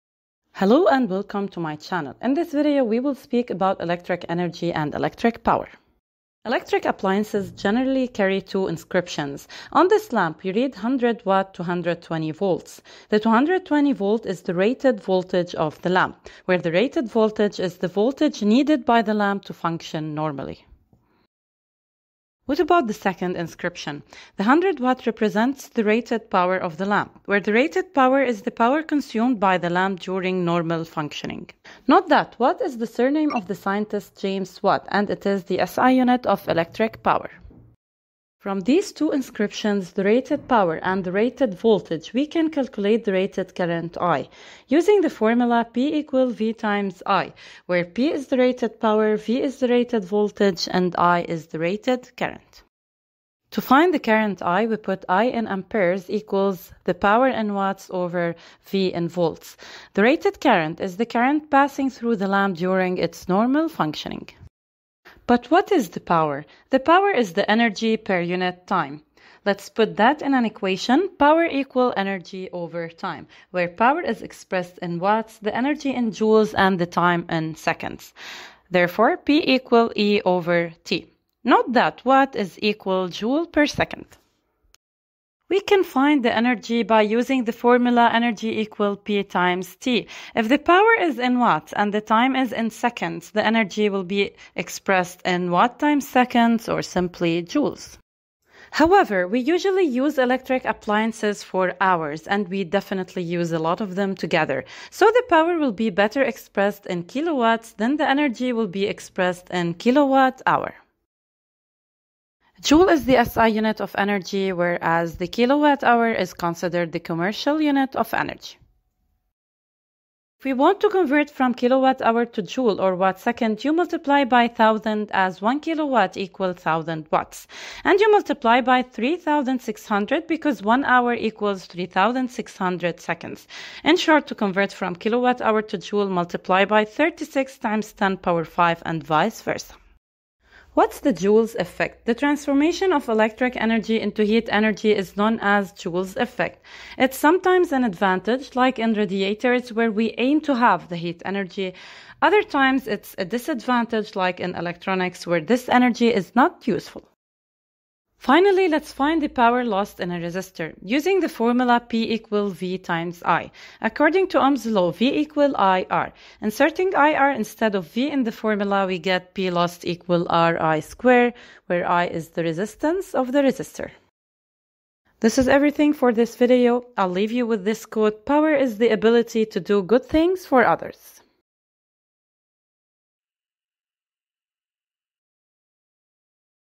Electric energy & power G9(animation)
In this video we speak about electric enery and electric power.we also speak about Joule's effect.This lesson is from grade 9 curriculum